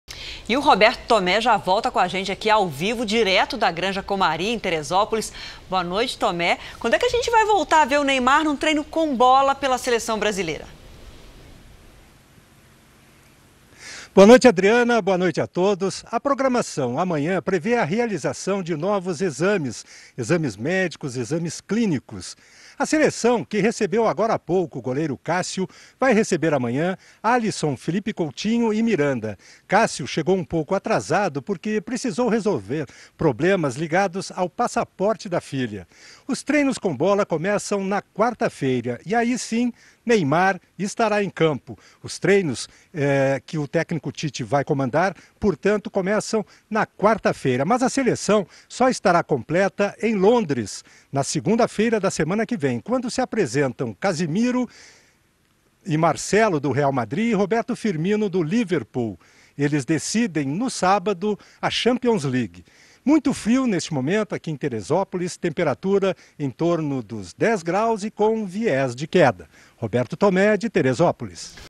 Neymar começa os treinos com a seleção brasileira na quarta (23)
No entanto, a seleção brasileira só estará completa na próxima segunda-feira (28).